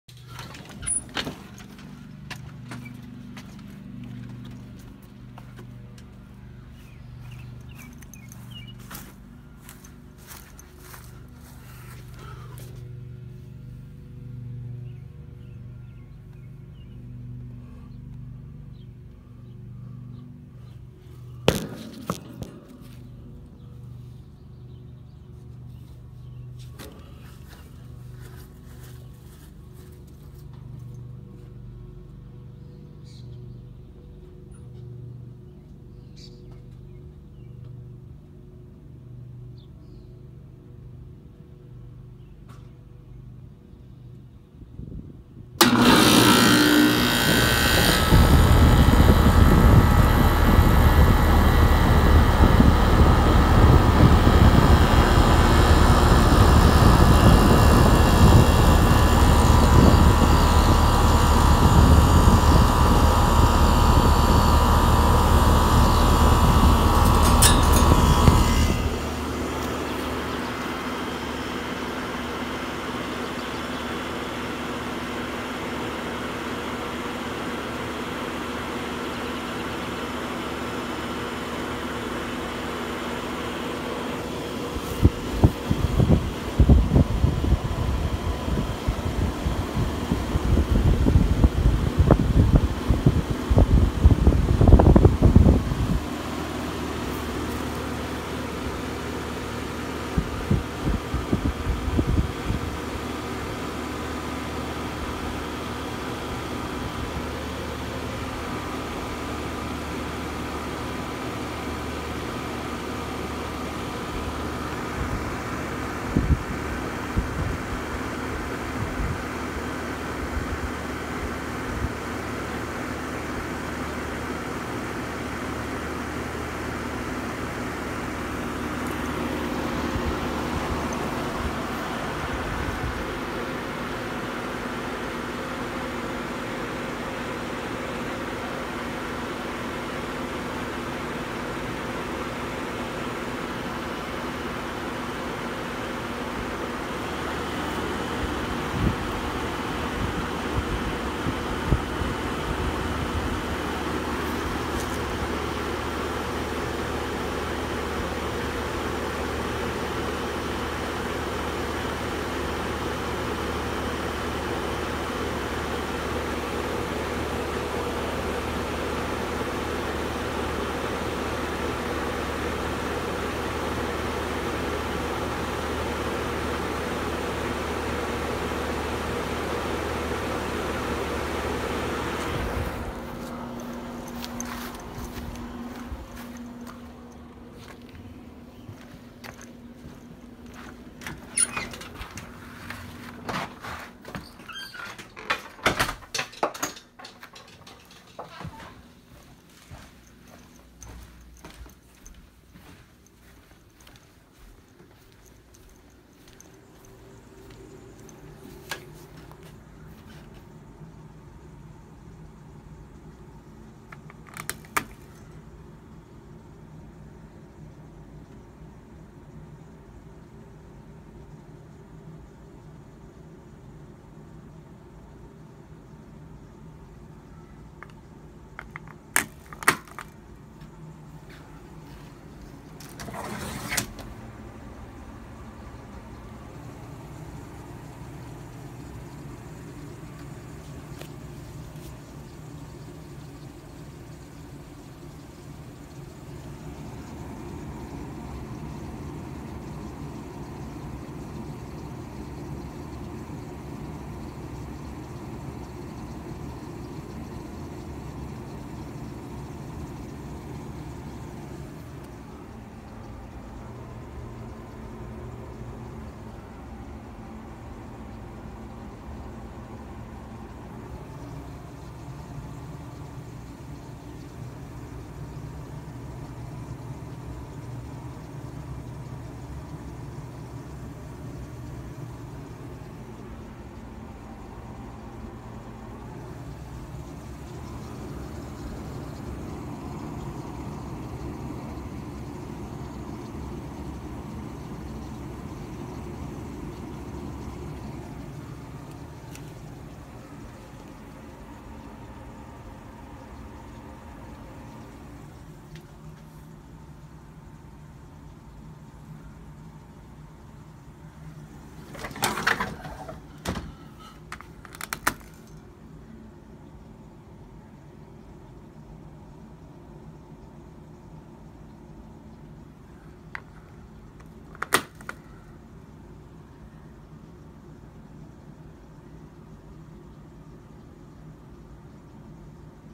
My Ruud Air Conditioner Starting Up 5/8/17
Wasn't the First start for the year, Recored on IPhone 7 plus